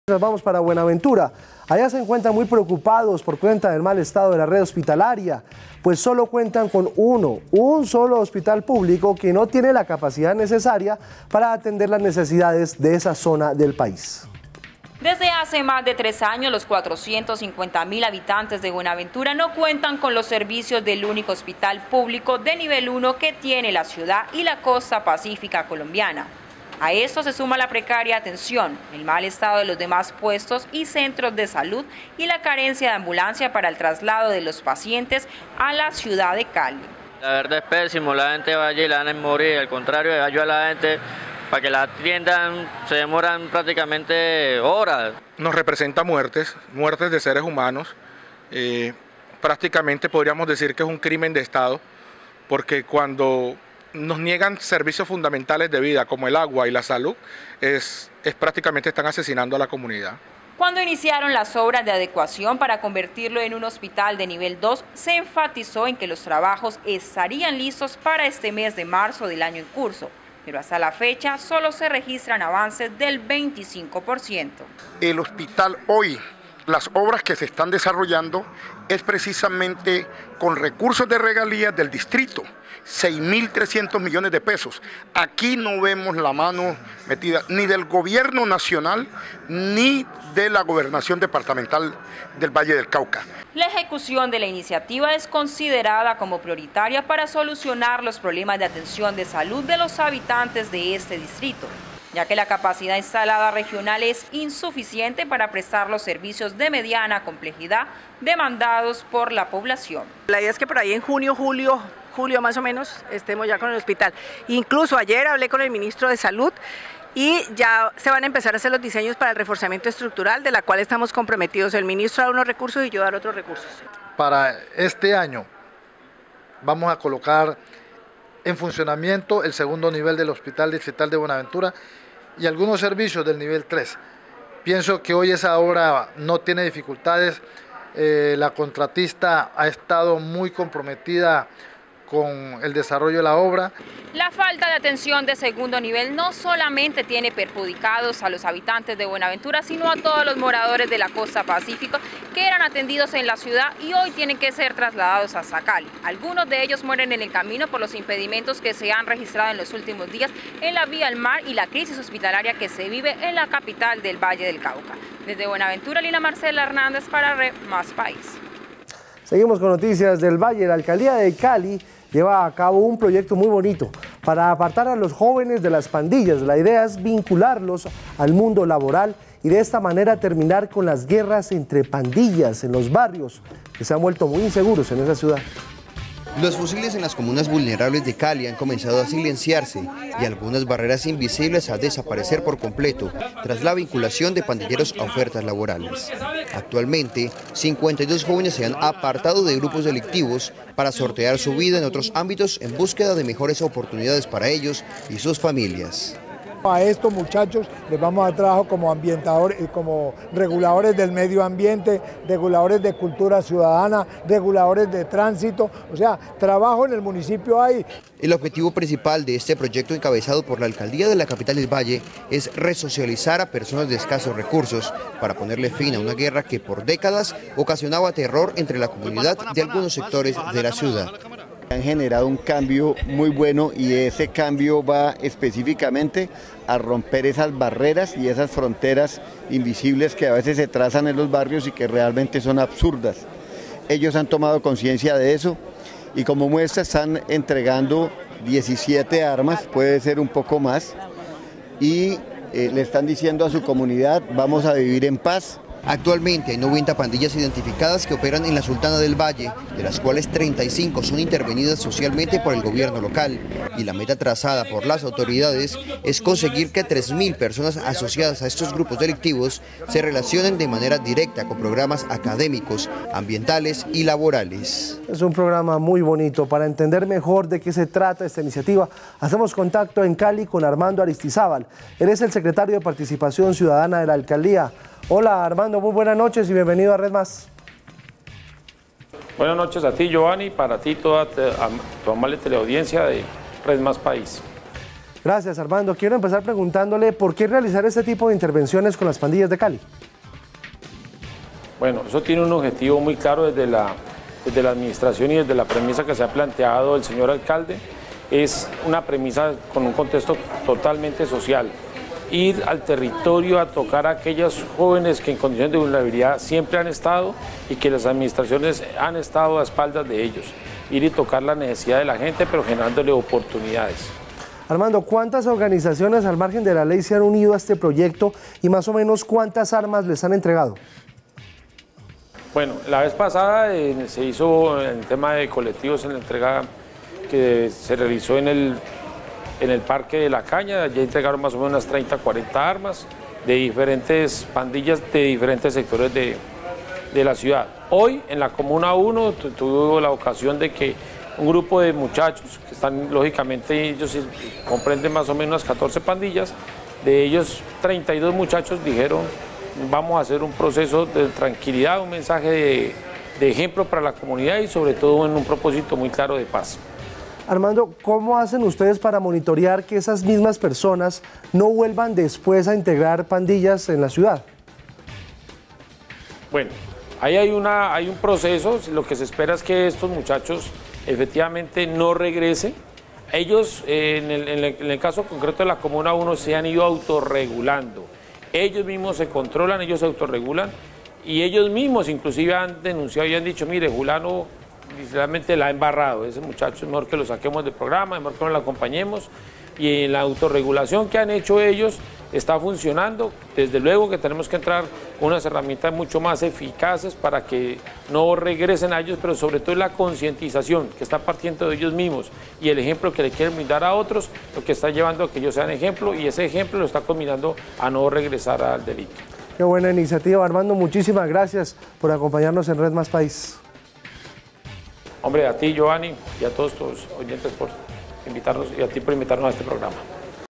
Alcaldía de Cali busca acabar la guerra entre pandillas ofreciendo trabajo
La alcaldía de Cali lleva a cabo un proyecto para apartar a los jóvenes de las pandillas, la idea es vincularlos al mundo laboral y de esta manera terminar con las guerras en los barrios más inseguros de la ciudad...